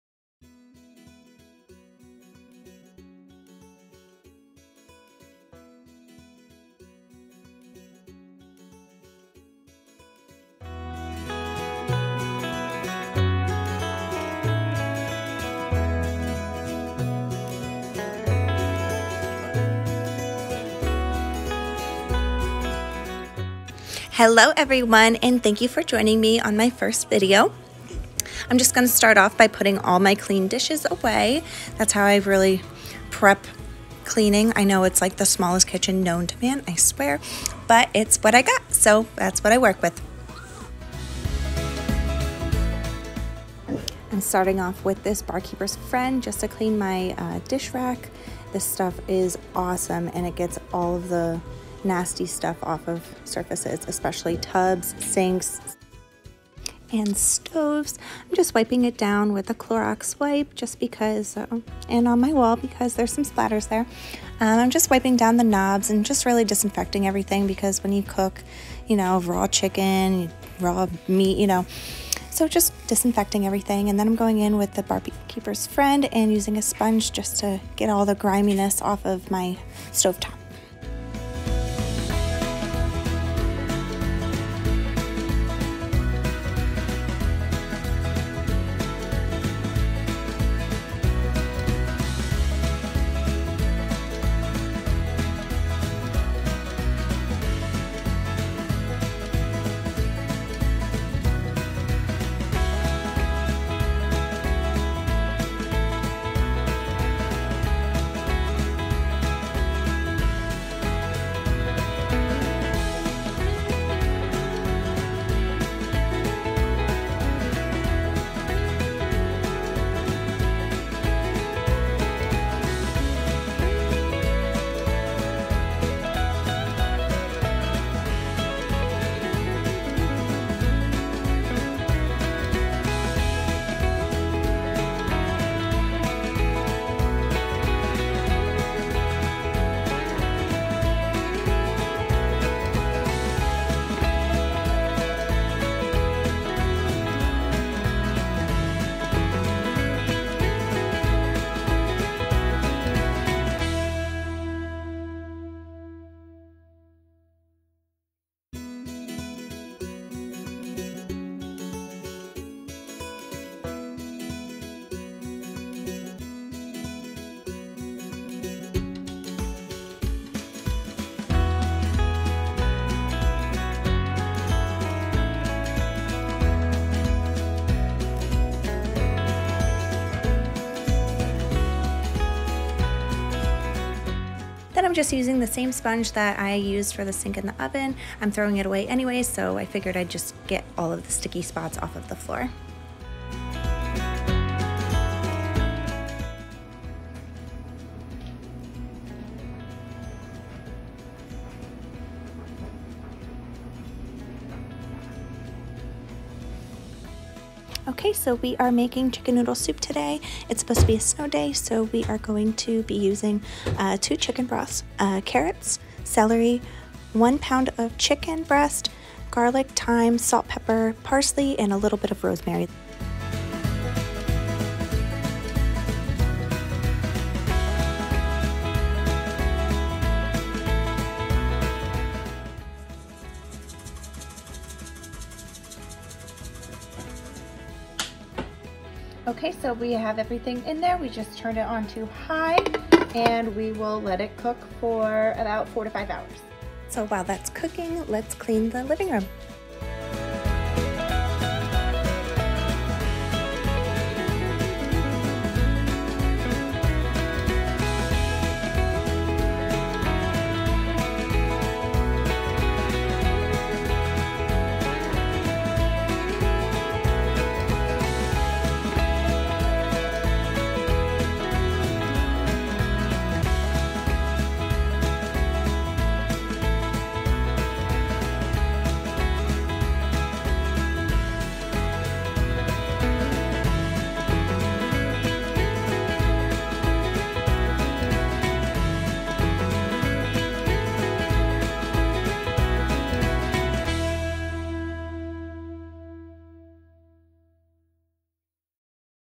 A DAY IN THE LIFE / DITL / CLEANING MOTIVATION / SMALL KITCHEN CLEANING
Back in March, my mom and I would video chat every day and if we were feeling unmotivated one of us would start cleaning or organizing. It always motivated the other one to start cleaning immediately. I hope this video helps you find motivation!